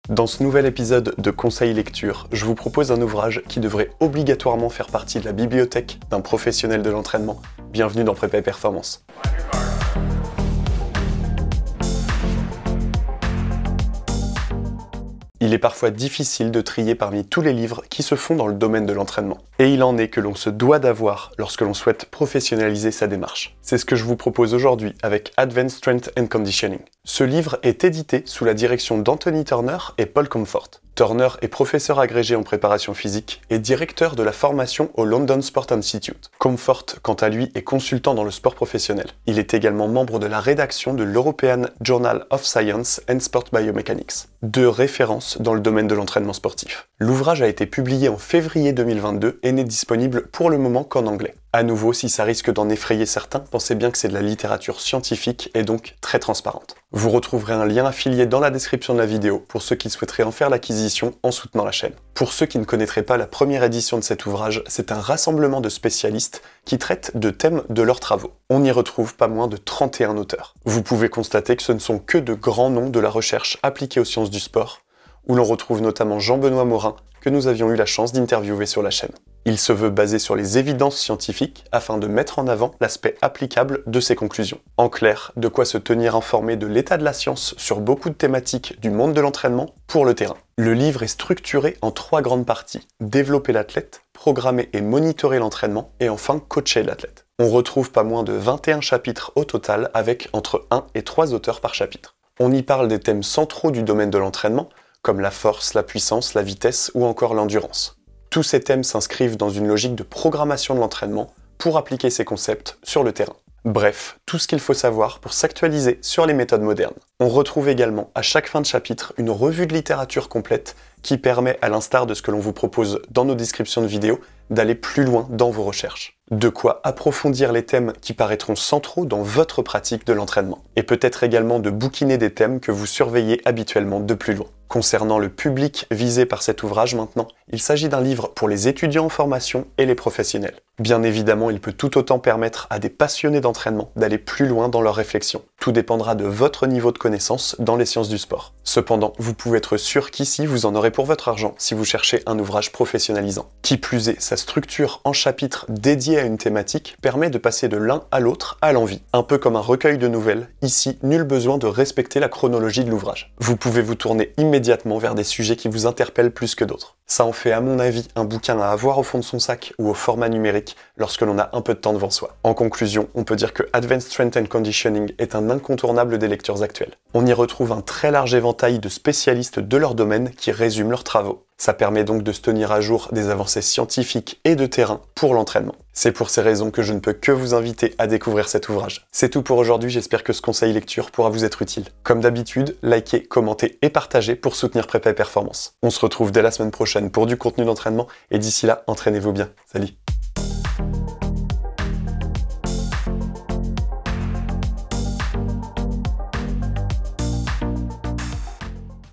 Conseil lecture #5 : Advanced Strength and Conditioning (Anthony Turner, Paul Comfort, Routledge)
📚Advanced Strength and Conditioning (Numérique)
📚Advanced Strength and Conditioning (Broché)

Titre : Advanced Strength and Conditioning
Sous la direction de : Anthony Turner & Paul Comfort
Editions : Routledge Handbook
Participation : Perry Stewart, Richard Blagrove, Anthony N. Turner, David Bishop, Richard Clarke, Rodrigo Aspe, Johnathan Hughes, Shyam Chavda, Vince Kelly, Patrick Holmberg, David Jenkins, Mark Russell, Natalie Brown, Samuel P. Hills, Liam P. Kilduff, John J. McMahon, G. Greg Haff, Mellissa Harden, Louis Howe, Chris Bishop, Timothy J. Suchomel, Christopher J. Sole, Christopher R. Bellon, George Beckham, Tom Dos’Santos, Paul Jones, Pedro Jimenez-Reyes, Jean-Benoît Morin, Nick Winkelman, Brian Gearity and Christopher Szedlak.